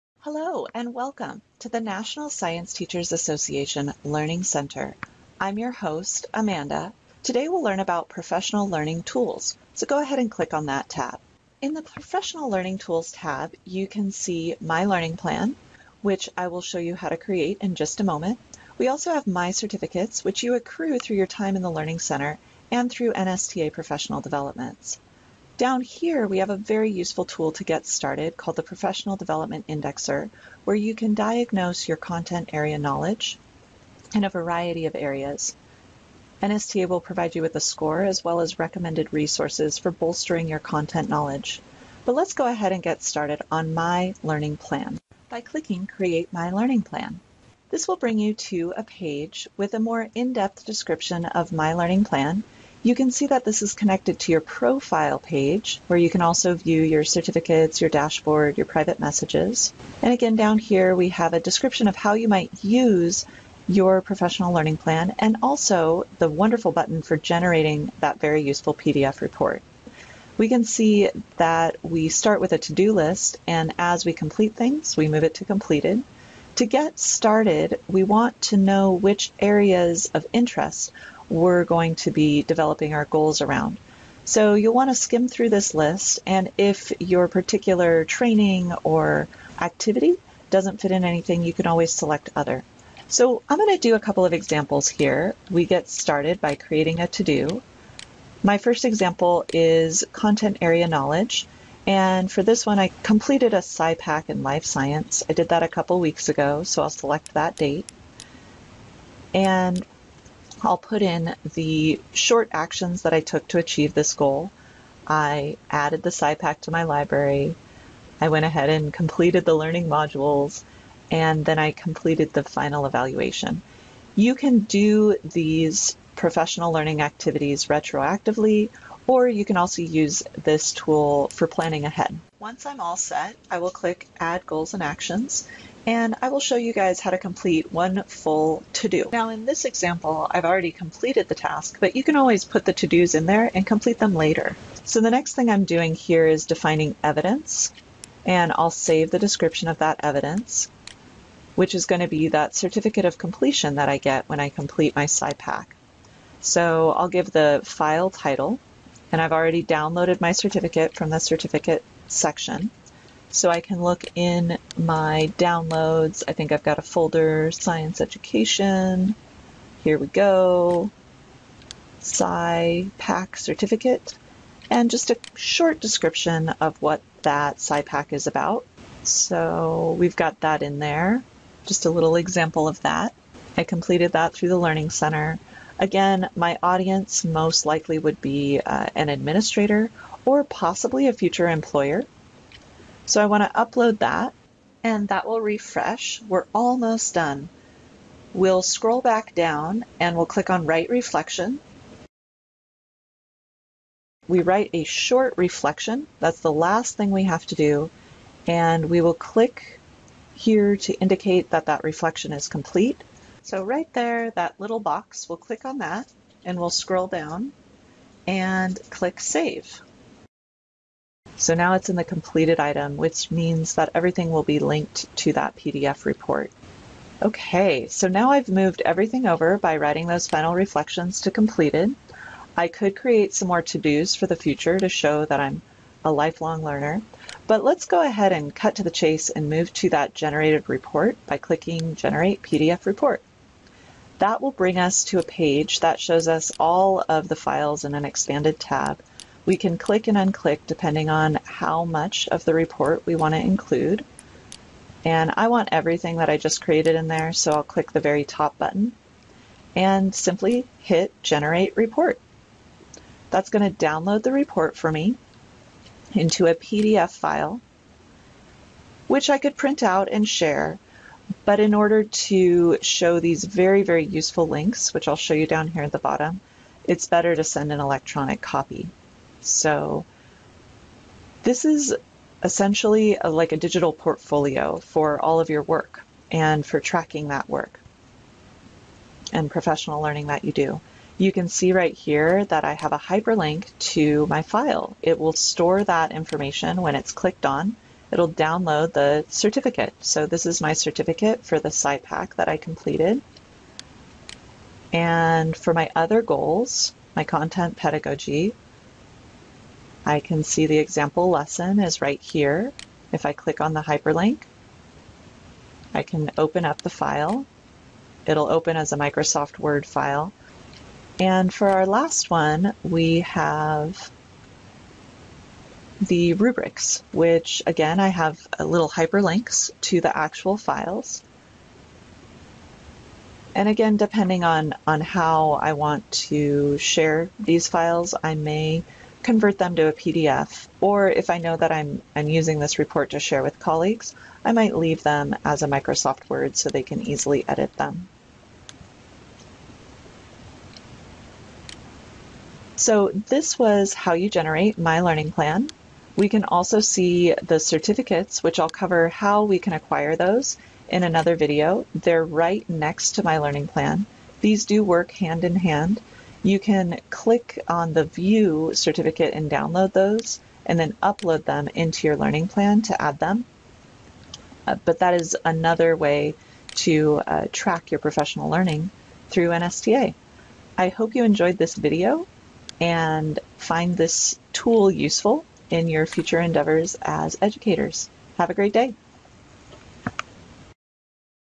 How to Make a Professional Learning Plan
NSTA's eLearning Engagement Specialist, Amanda Wolfe, shows you how to make a professional learning plan in the NSTA Learning Center.